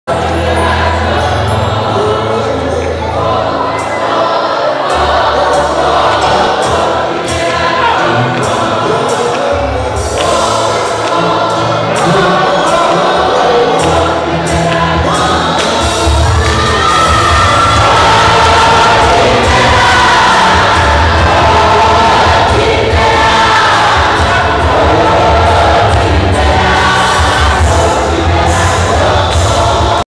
Bantu Congregational Church Of Zion In RSA(Bishop LL Makasi)
Prof Rt Rev JJR Makasi singing at the 60th Anniversary which was his last one. We will always miss him.